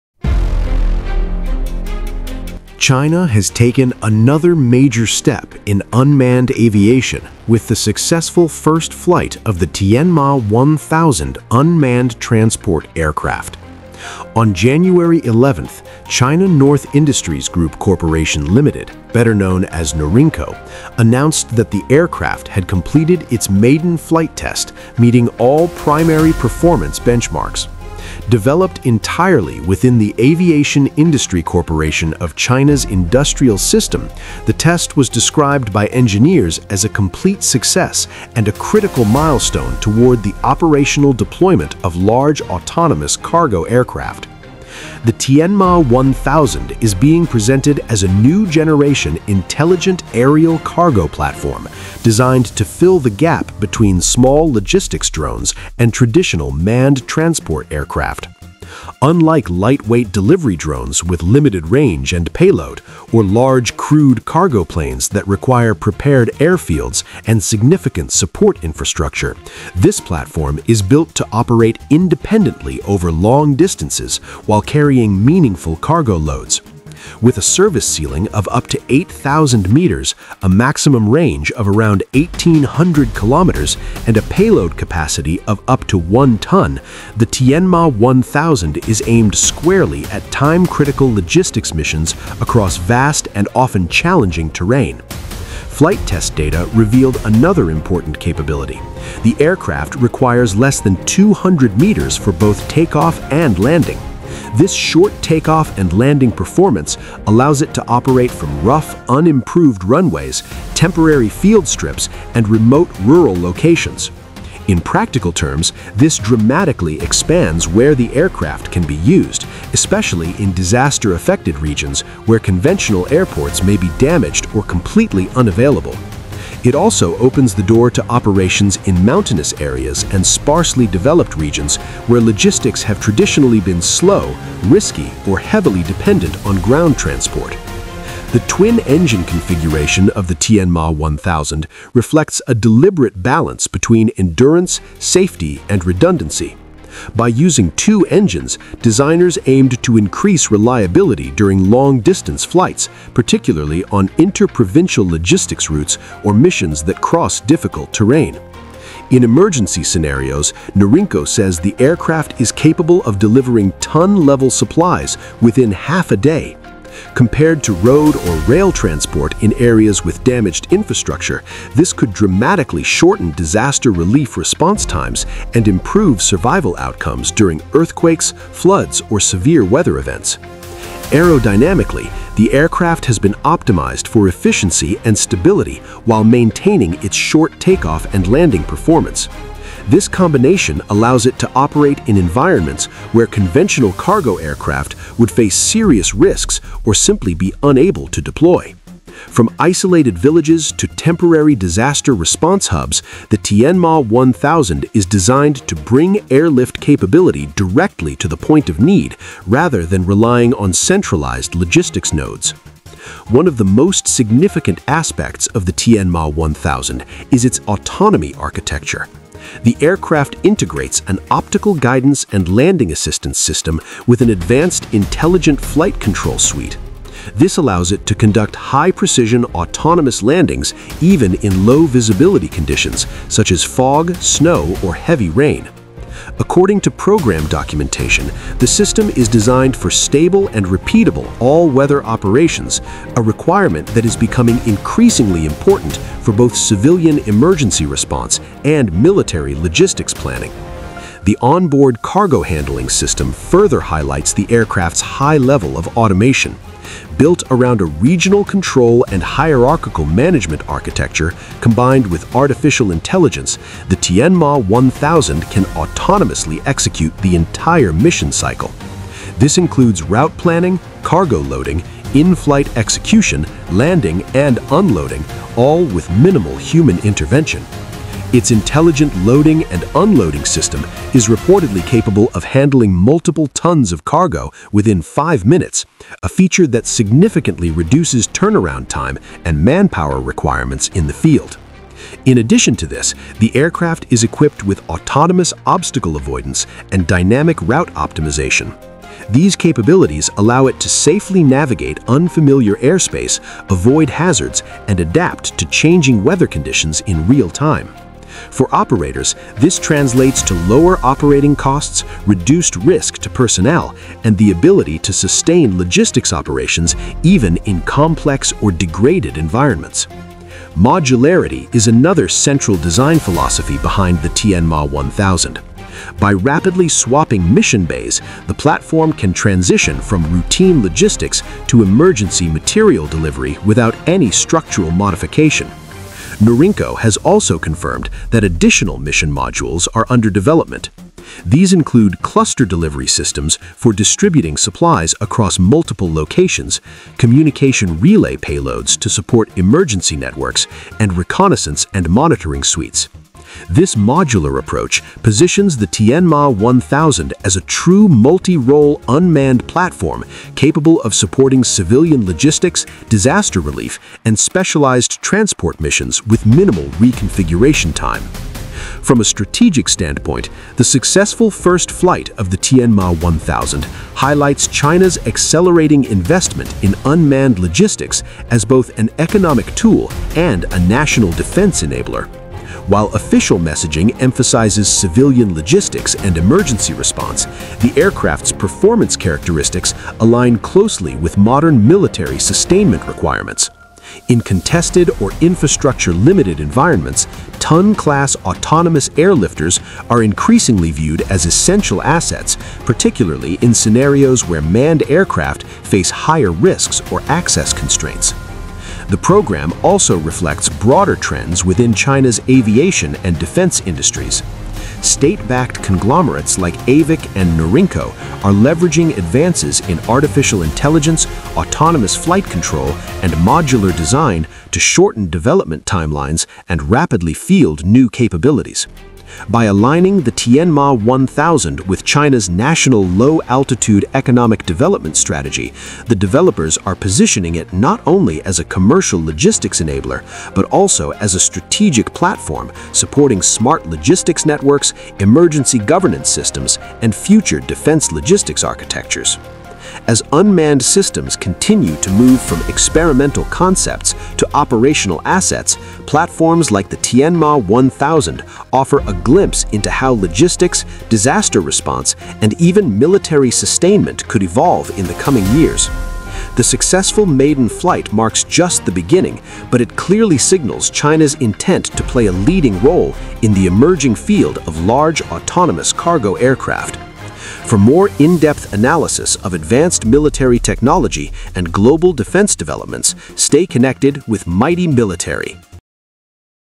China’s Tianma-1000 Cargo Drone Completes First Flight
China has successfully conducted the first flight of its new Tianma-1000 unmanned transport aircraft, marking a major breakthrough in autonomous logistics aviation. Announced by China North Industries Group Corporation Limited (NORINCO) on January 11, the aircraft met all key performance benchmarks during its maiden sortie and is now being positioned for future operational deployment.

Designed within the Aviation Industry Corporation of China (AVIC) industrial system, the Tianma-1000 bridges the gap between small logistics drones and traditional manned cargo aircraft. With a payload capacity of up to one ton, a range of 1,800 kilometers, and a service ceiling of 8,000 meters, the platform is optimized for time-critical logistics missions across vast and challenging terrain.

One of its most notable features is its short takeoff and landing capability, requiring less than 200 meters of runway. This allows the aircraft to operate from rough, unimproved strips, temporary landing zones, and disaster-affected areas where conventional cargo planes cannot safely operate. In emergency scenarios, the Tianma-1000 can reportedly deliver ton-level supplies within half a day, dramatically improving disaster response timelines.

The aircraft also features advanced autonomy, including optical guidance, intelligent flight control, all-weather autonomous landing, and AI-driven route optimization. Its automated cargo handling system enables rapid loading and unloading with minimal manpower, while modular mission bays allow the platform to quickly switch roles between logistics, emergency delivery, communications relay, and reconnaissance missions.

Although officially framed as a civilian logistics and disaster-relief platform, the Tianma-1000’s capabilities closely align with modern military sustainment needs. The successful first flight highlights China’s accelerating push toward unmanned logistics systems that can operate in contested or infrastructure-limited environments.

This video is for educational and informational purposes only. We do not claim ownership of any third-party content.